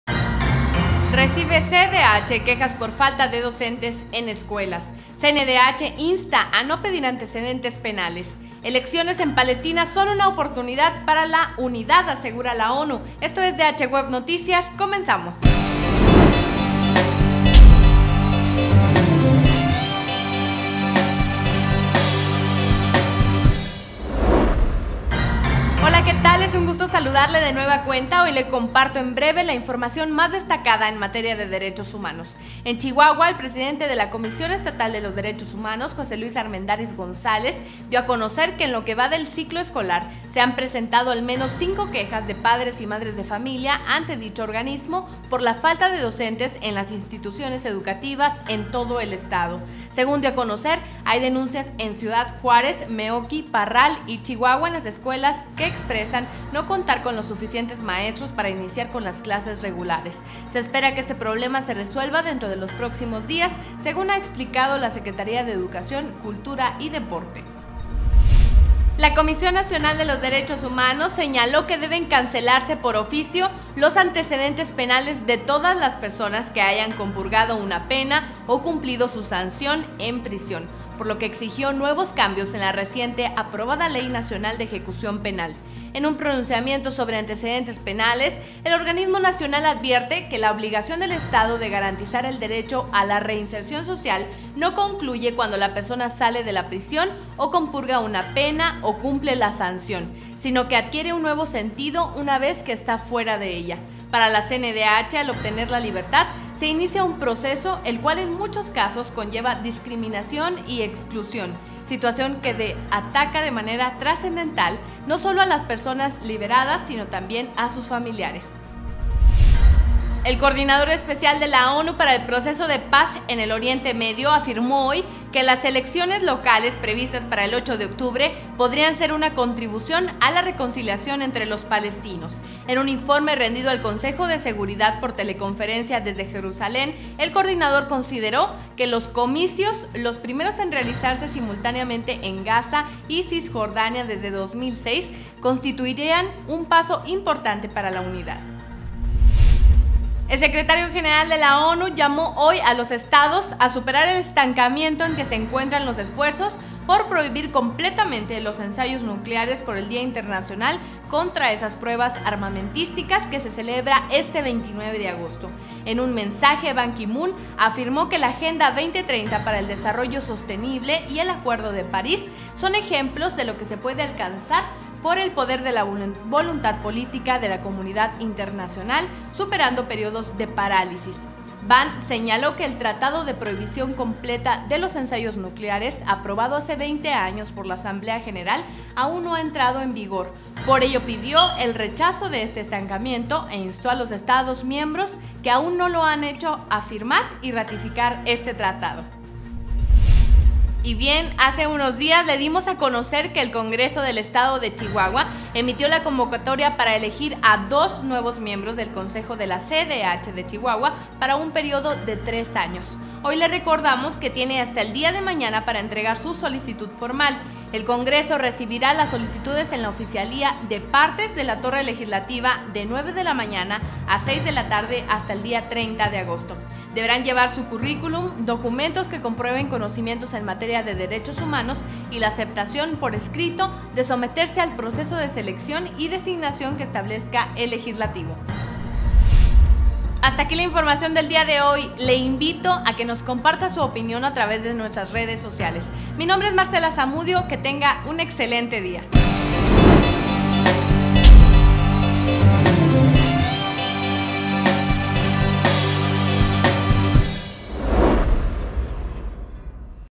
CNDH insta a no pedir antecedentes penales
• Recibe CEDH quejas por falta de docentes en escuelas

• Elecciones en Palestina son una oportunidad para la unidad: ONU

• ONU pide a naciones ratificar la Prohibición Completa de Ensayos Nucleares